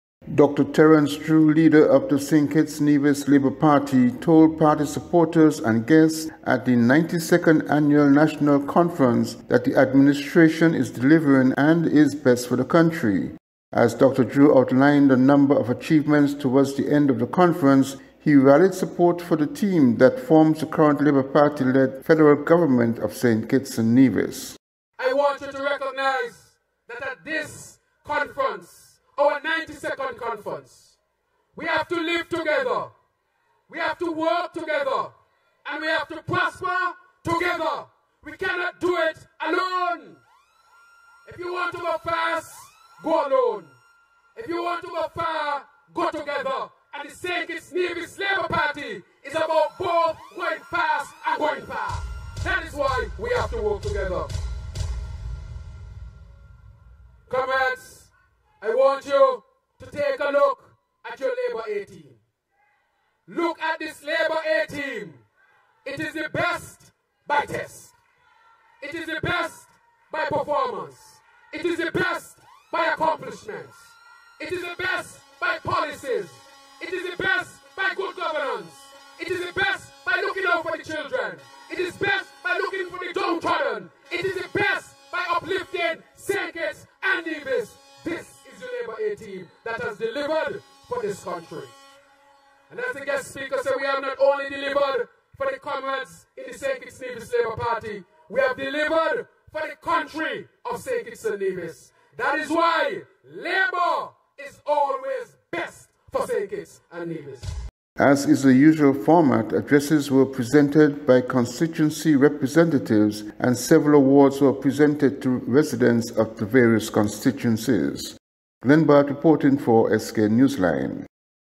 LABOUR LEADER RALLIES FOR TEAM AT 92ND CONFERENCE
Dr Terrance Drew, Leader of the St. Kitts-Nevis Labour Party told party supporters and guests at the 92nd Annual National Conference that the administration is delivering and is best for the country.
As Dr Drew outlined a number of achievements towards the end of the conference, he rallied support for the team that forms the current Labour Party led Federal Government of St. Kitts and Nevis.